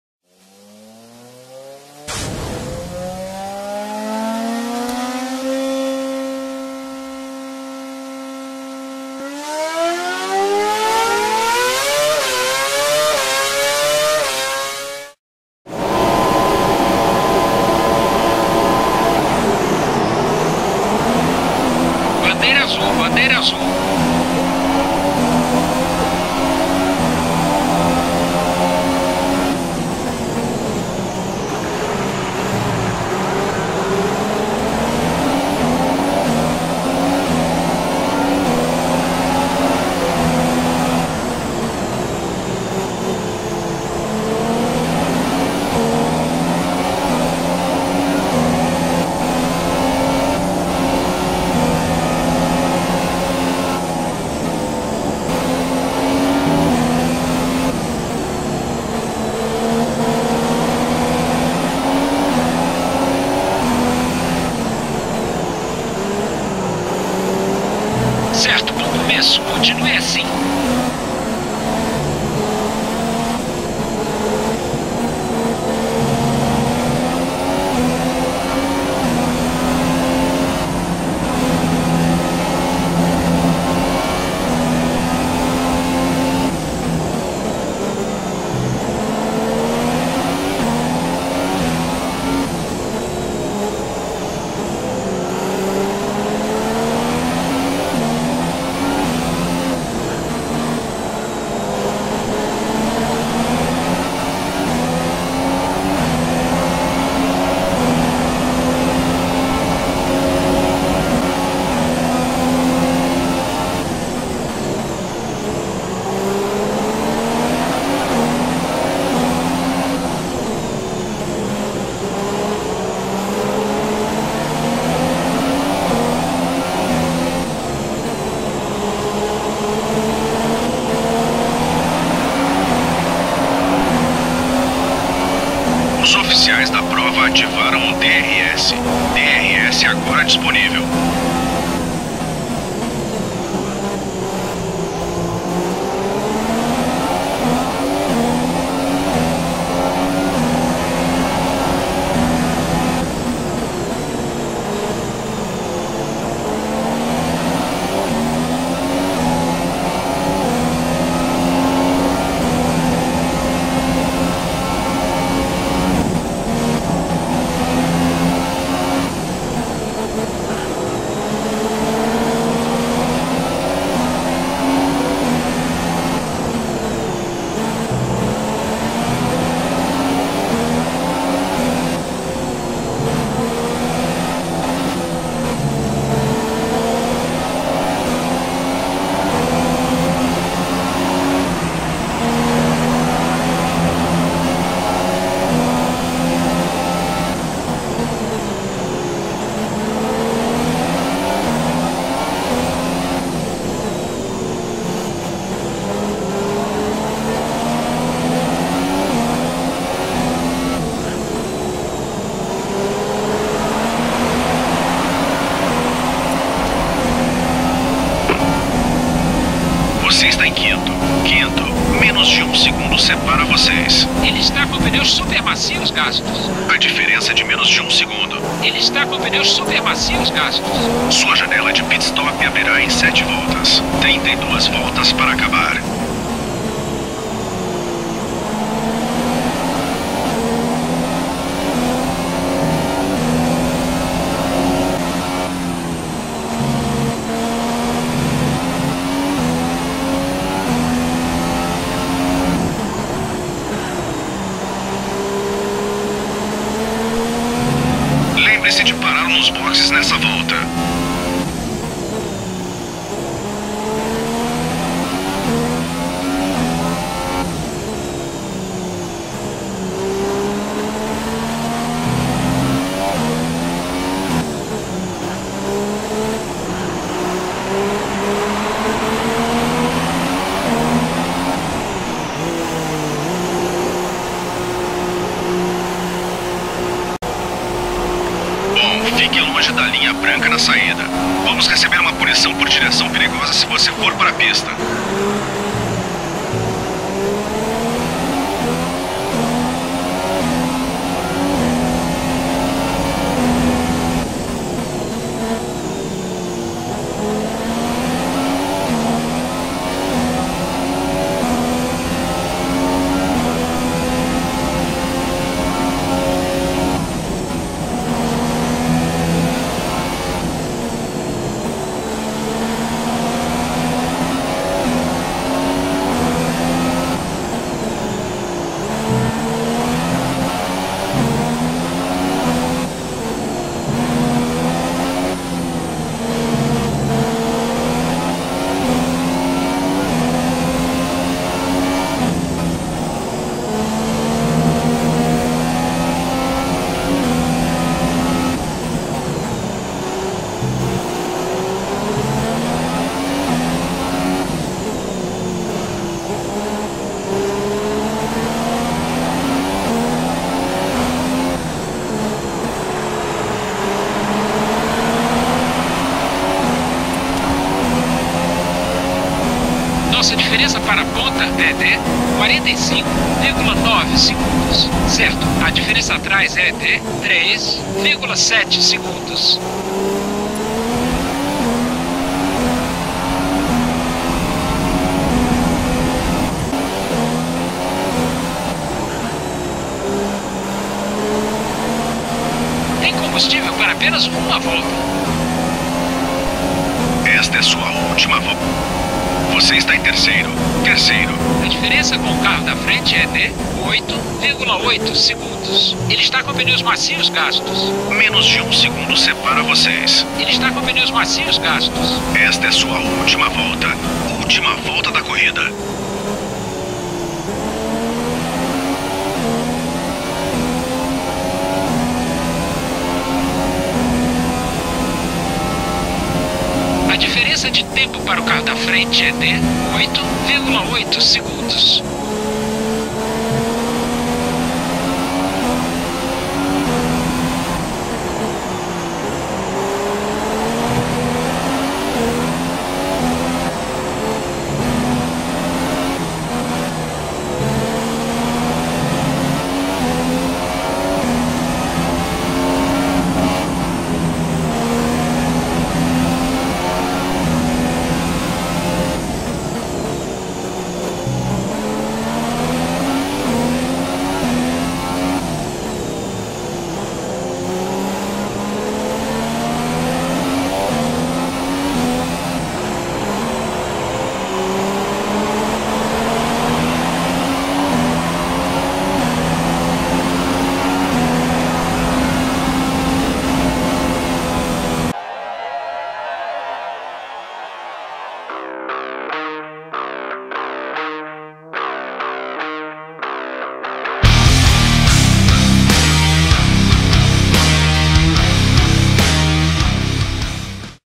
GUSTAVO COSTA - Melhores Momentos (GP HUNGRIA)
LOF1_GustavoCP

Quarta etapa do campeonato online de F1 pela Liga Online F1, no GP da Hungria.

Largada: 6º
Chegada: 3º
Pneus: Super Macio / Macio